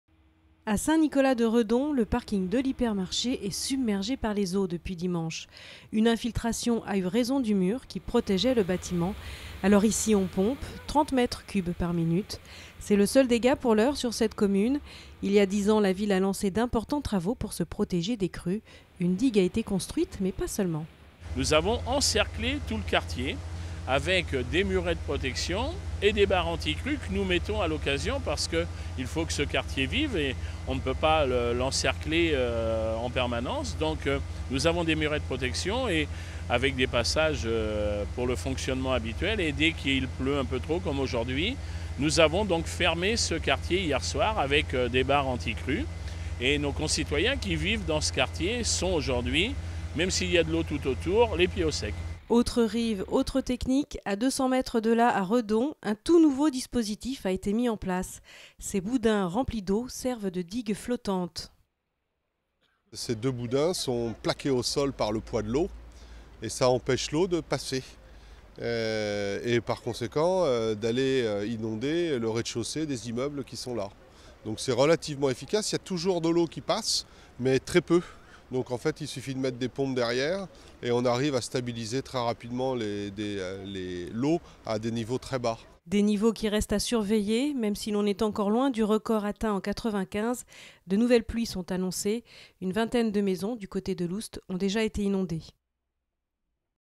Face à la Vilaine en crue, Redon et Saint-Nicolas s'organisent
Comment lutter contre la montée des eaux dans le bassin de la Vilaine ? Redon a opté pour les boudins géants, et Saint-Nicolas pour les murets amovibles.

Reportage de Clémentine Gardet pour Télénantes.